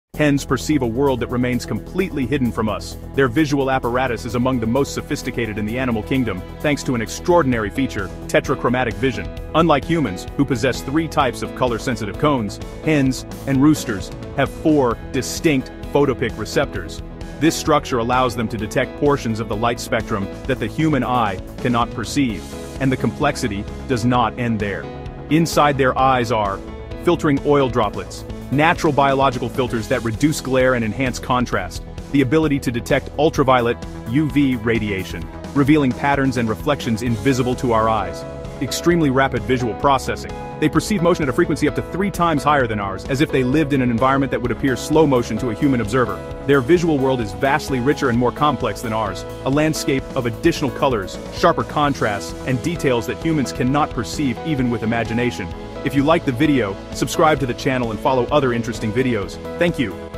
The Hen: A Visual System Superior to Our Own
Discover the extraordinary world of hens and their incredible tetrachromatic vision! 🐔🌈 Unlike humans, who see through just three color receptors, hens possess four! This remarkable ability allows them to perceive a vibrant spectrum of colors and details hidden from our eyes. In this fast-paced YouTube Short, we’ll explore how this sophisticated visual system gives hens a unique perspective on their environment. From the way they navigate their surroundings to their interactions with each other, you’ll be amazed by what they see!